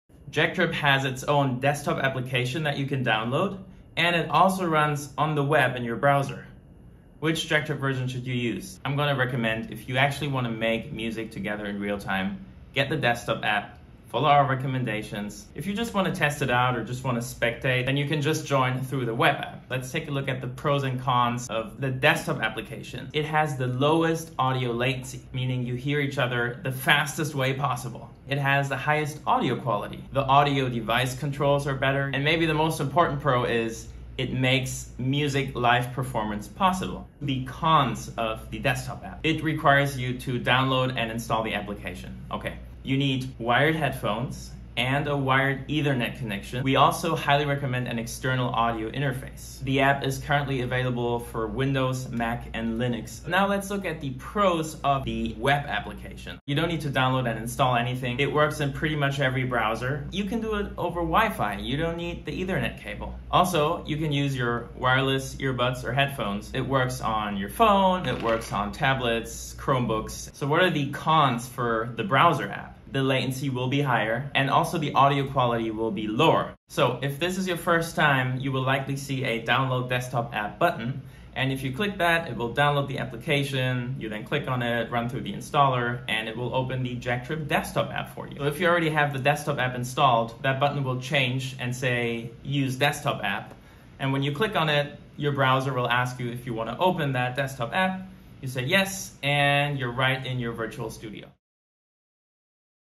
Desktop App vs. Browser Web App - Get Started with JackTrip
JackTrip now also runs in the browser! What is the difference between the Web App and the Desktop App? Here's a quick comparison!

Find more JackTrip tutorials on our YouTube page - we'd love your feedback!